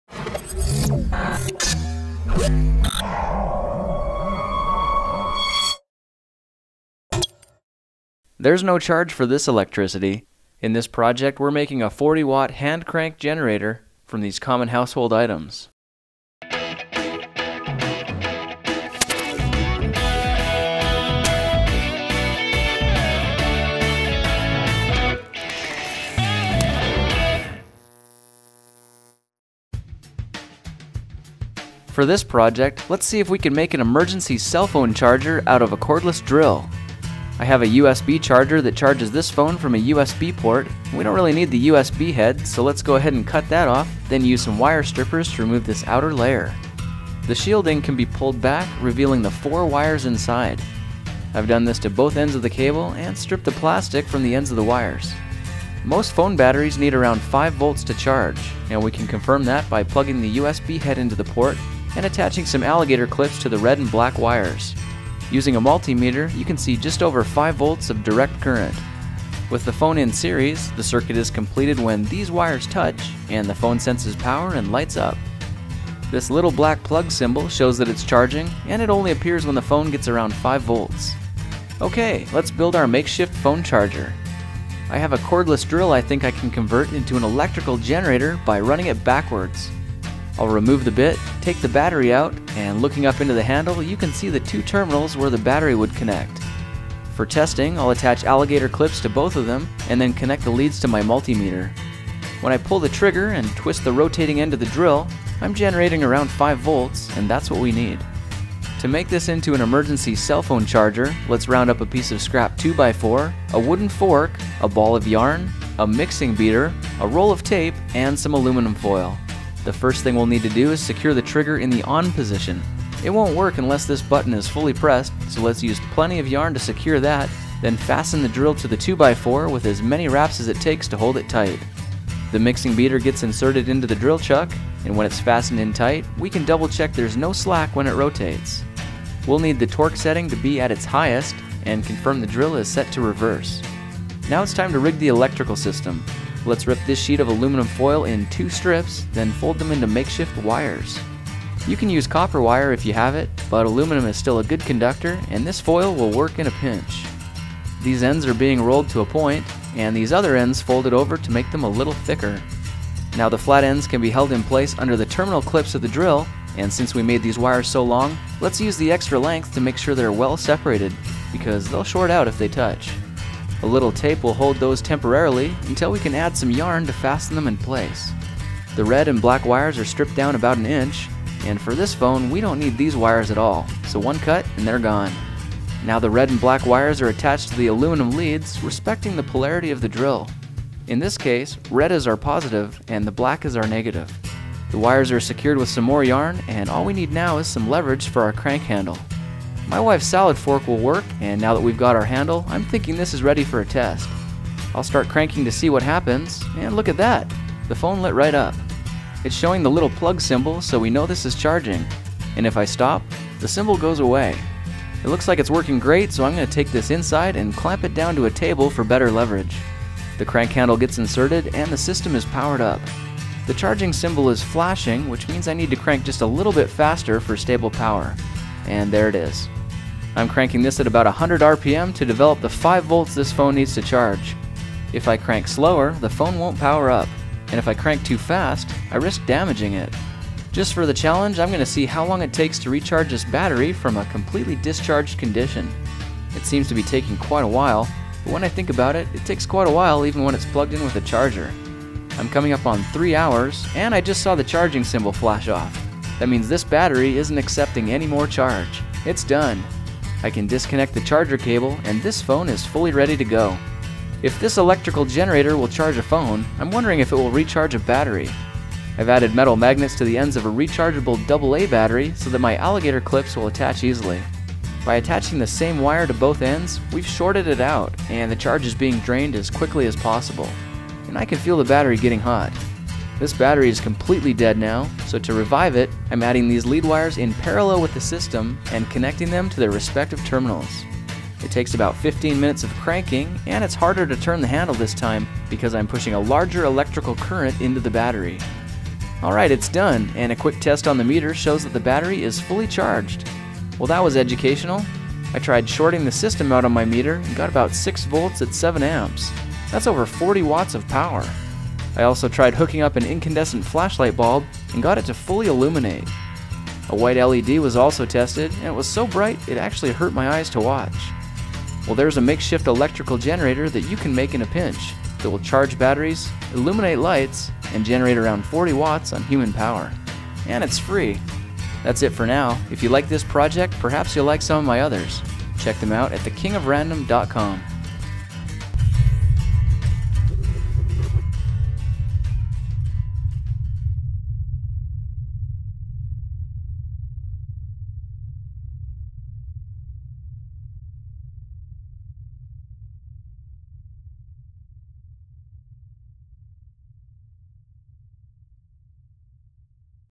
Make an Emergency Phone Charger - MacGyver Style!
In this video, you'll learn how to "MacGyver" a 40 Watt Electrical Generator from a cordless drill and a few household items.  Here's how to charge your phone, illuminate small lights, and make electricity in a pinch.

WARNING:

This project is intended to be a "bare bones" approach to generating electricity in a tight situation.  There are no voltage regulators, no diodes and no capacitors to smooth the current.  There may a risk in charging lithium-ion batteries without a proper battery charging circuit recommended by the manufacturer.  It worked fine for me, but if you try this on your phone, make sure you understand, and are comfortable, with the risks.  Back up your data in case your phone is adversely affected and your data or equipment is damaged as a result.  Use of this video content is at your own risk.By running an electric motor backwards, you can generate electricity.

When using a cordless drill, remove the battery pack so you're not fighting the back EMF, and press the trigger button in to allow the electrical connection to be made.  Now all you have to do is twist the rotating end of the drill, and you'll be generating electricity at the contact points where the battery would normally connect.  On my drill, a cranking speed of 100 RPM yielded about 5 volts DC.

A few household items will make it practical to use.  For example, a crank handle (made from a mixing beater and a salad fork) will give better leverage for cranking.  Some aluminum foil that's been rolled up into a wire will help direct the electricity to the point of use.

The faster and harder you can crank the drill, the higher voltage, and more amperage you can extract.

Ideally, this could be hooked up to a bike, water power, or a windmill to generate effortless energy, and if done carefully, the energy could be stored in a battery for later use.